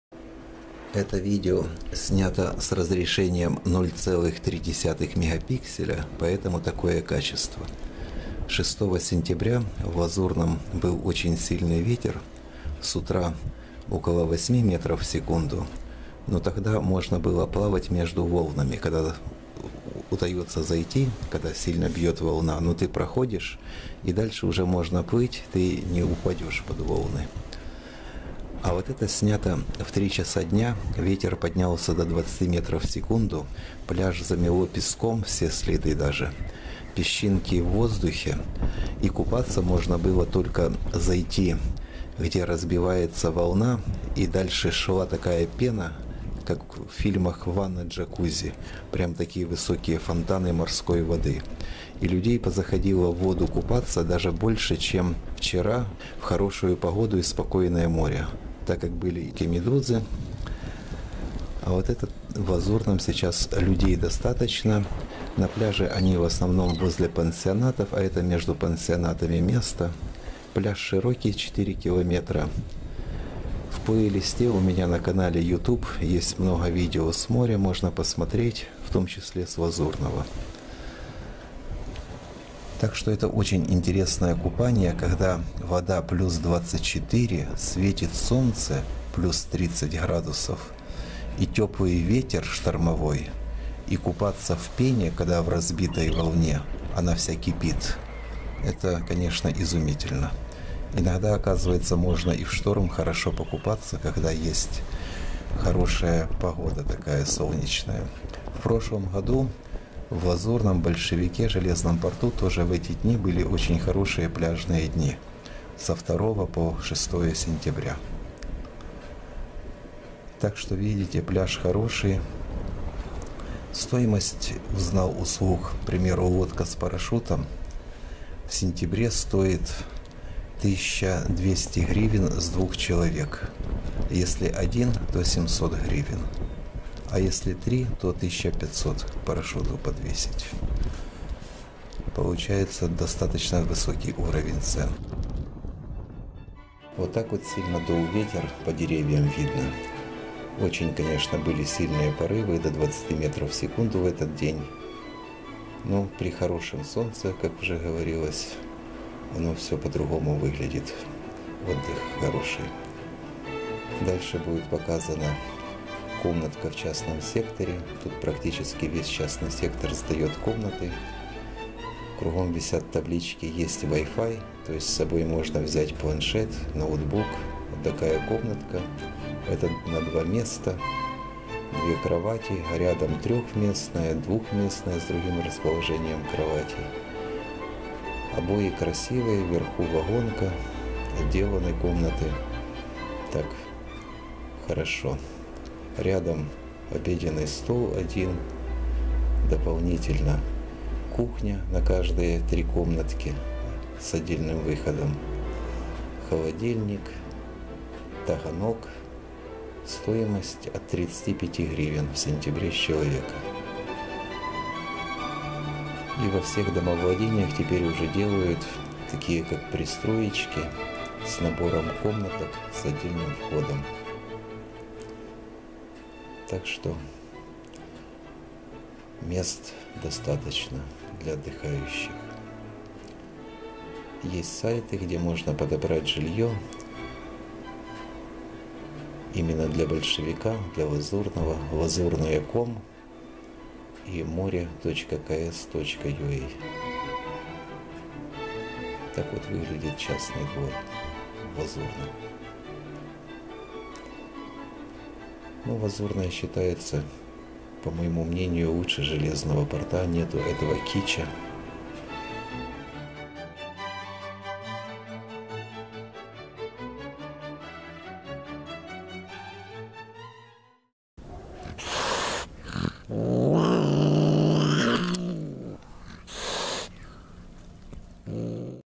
Лазурное шторм 6.09.2015 + цены на проживание. Хороший берег, песочек без камней
Видео снято 6 сентября 2015 года в Лазурном Херсонской области